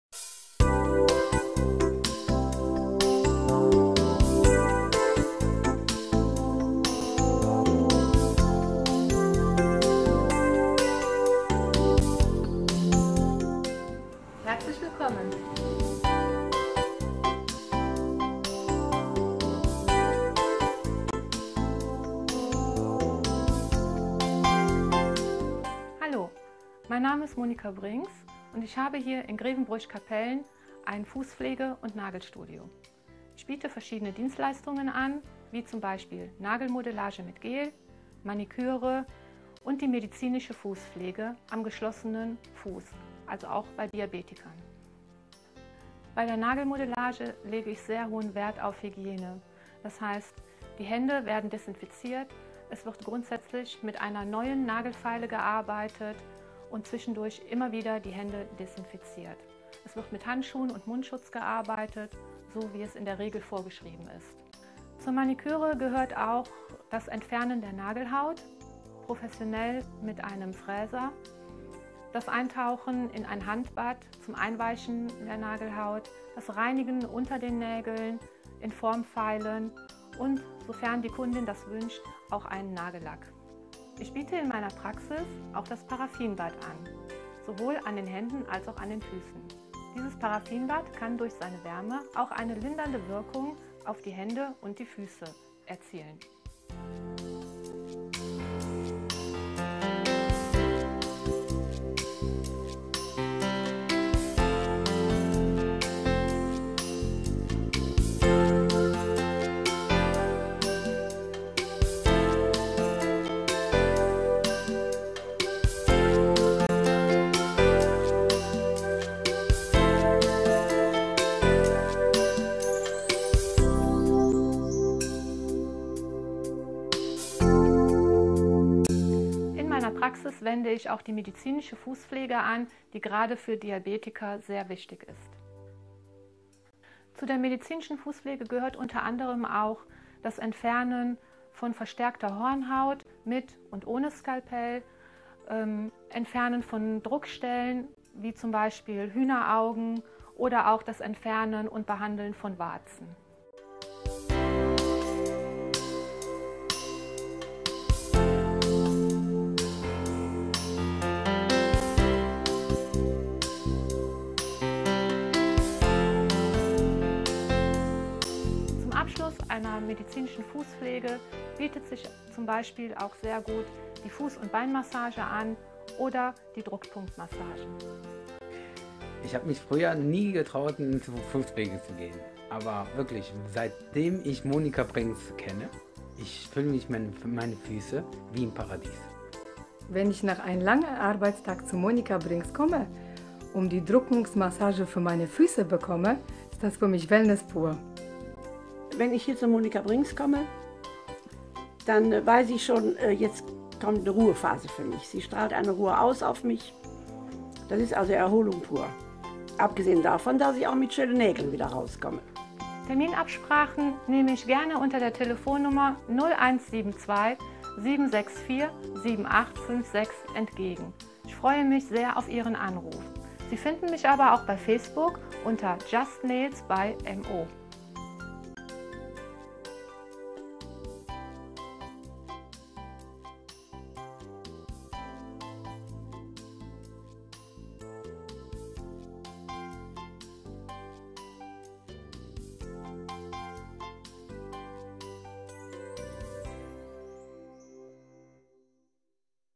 Monika Brings stellt sich vor in Kapellen an der Talstrasse
Monika stellt sich vor.